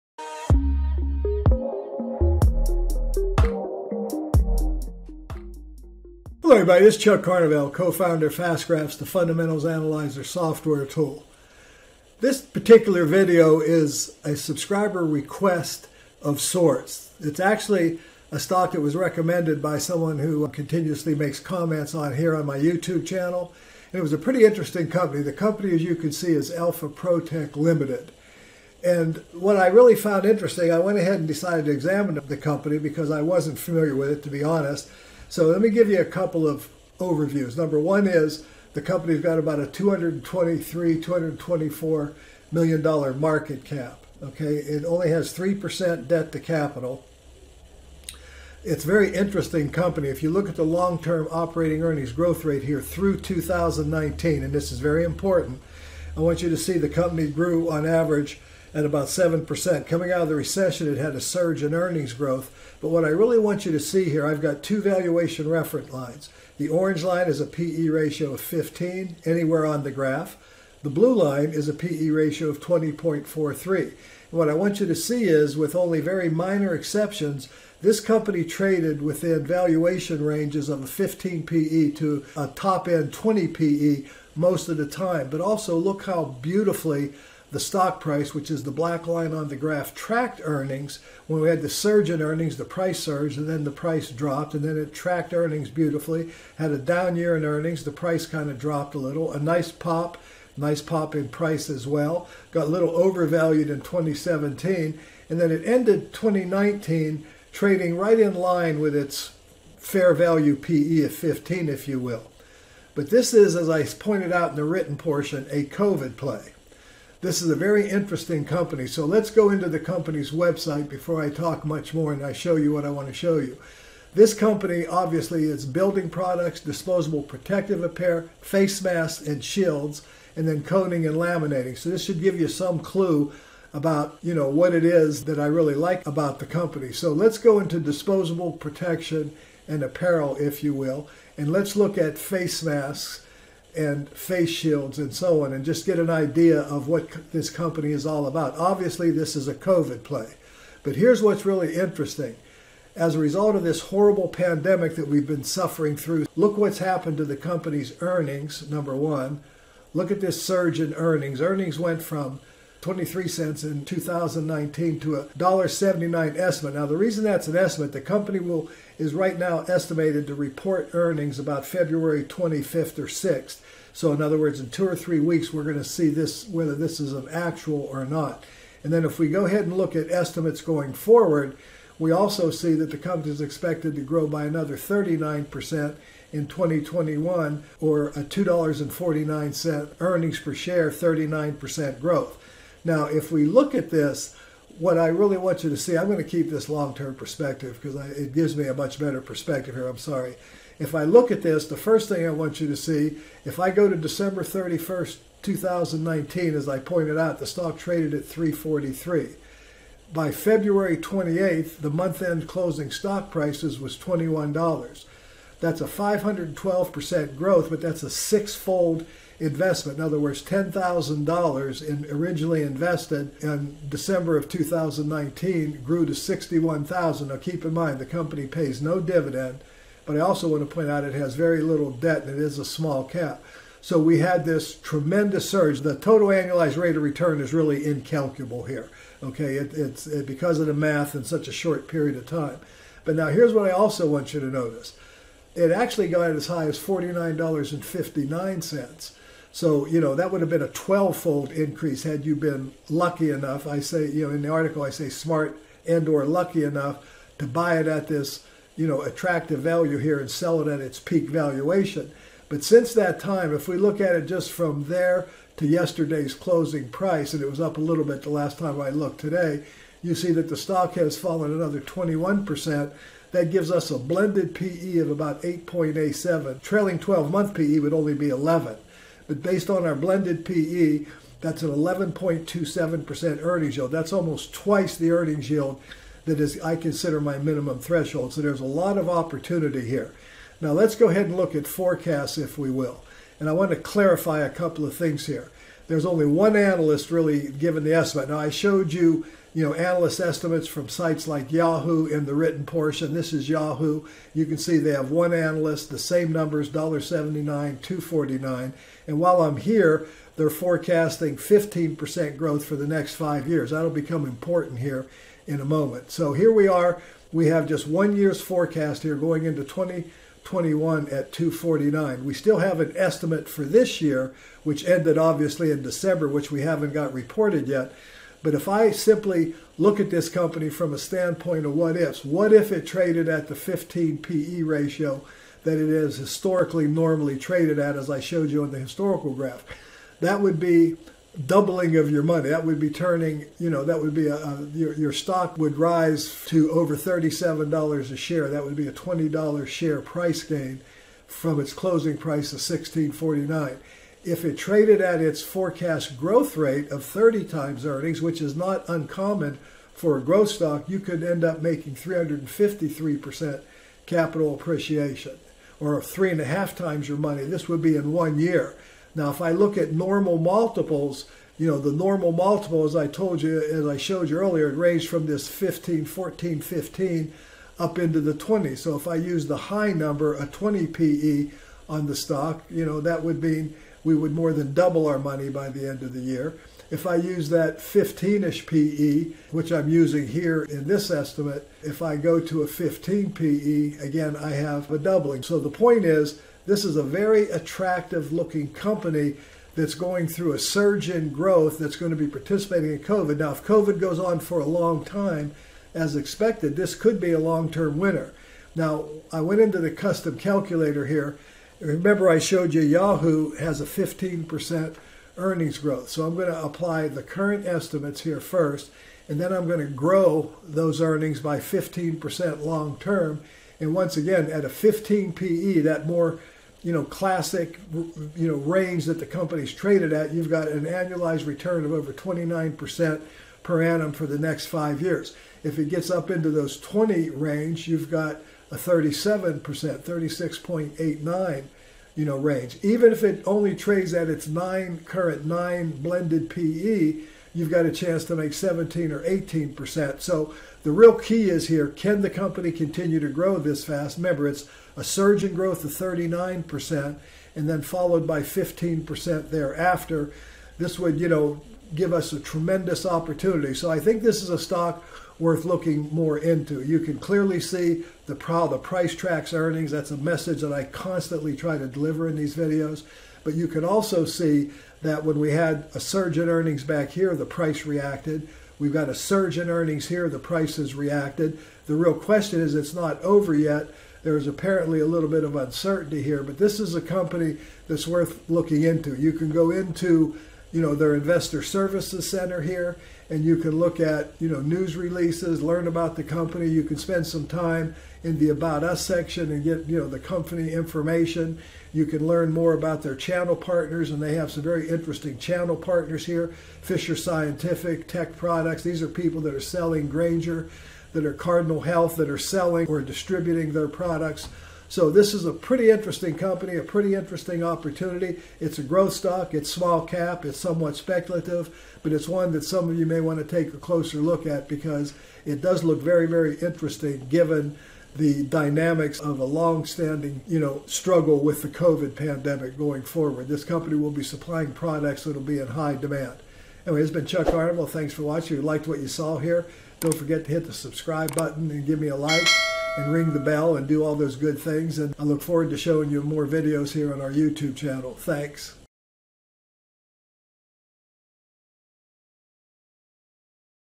This COVID-19 Play Could Double Or Triple Your Money In 2021: Alpha Pro Tech Limited | FAST Graphs
Alpha Pro Tech Limited, A Growth Opportunity
In the following video on Alpha Pro Tech, I elaborate more on the opportunity that this COVID-19 pandemic growth stock Alpha Pro Tech play offers for those interested in total return.  Clearly it is not a dividend paying stock.  Nevertheless, I found it was extremely important to illustrate that the market does provide opportunities to invest in high growth at reasonable values from time to time.  This is typically referred to as a GARP - or growth at a reasonable price - opportunity.  With growth stocks I am willing to pay more than the typical P/E ratio of 15.  When evaluating growth stocks I tend to rely on Peter Lynch’s famed formula P/E ratio equal to earnings growth rate.

The ARTICLE that goes along with this video on Alpha Pro Tech can be found here:

Try FAST Graphs for FREE Today!